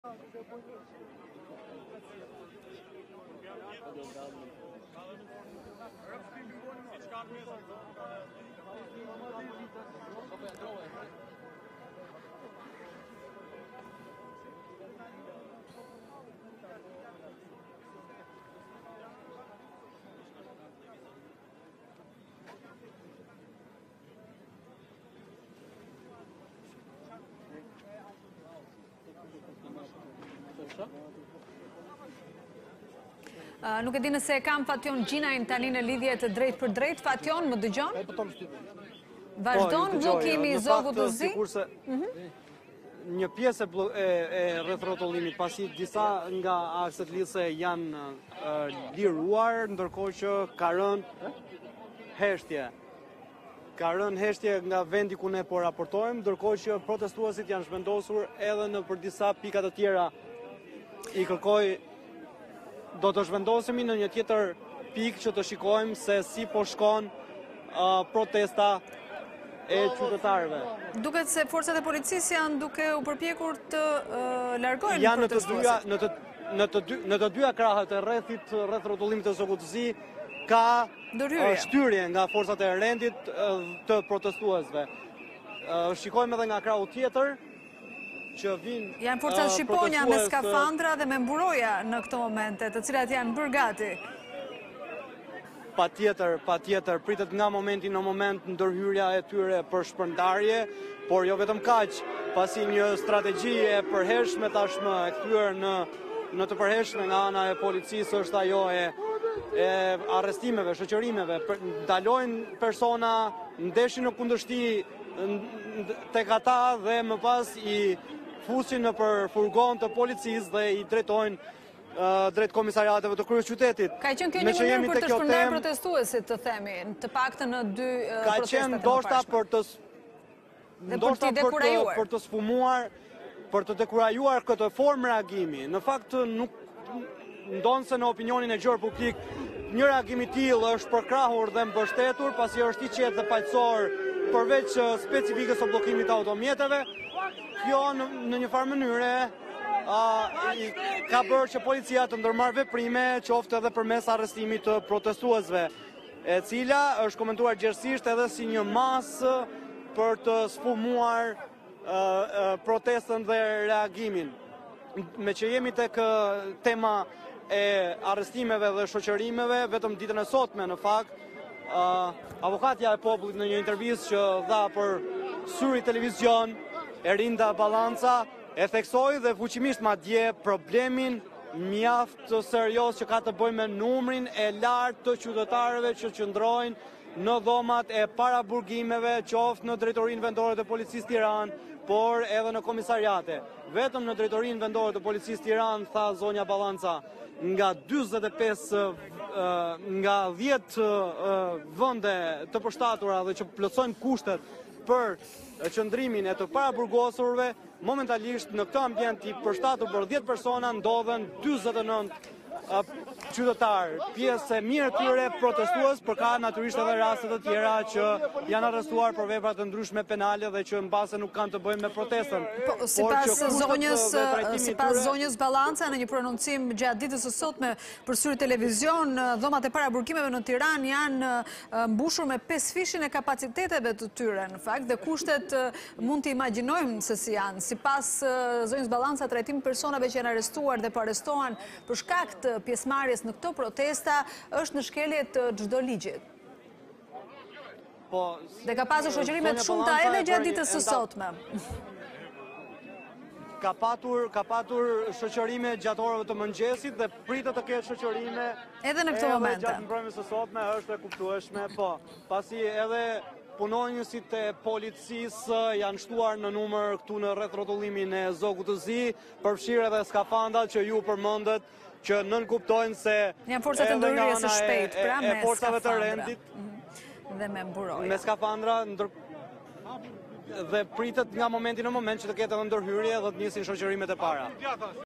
Direkt/'Zogu i Zi' blindohet nga policia
Rritja e çmimeve, qytetarët protestë në Tiranë, Lezhë, Shkodër, Laç, Mirditë dhe Ballsh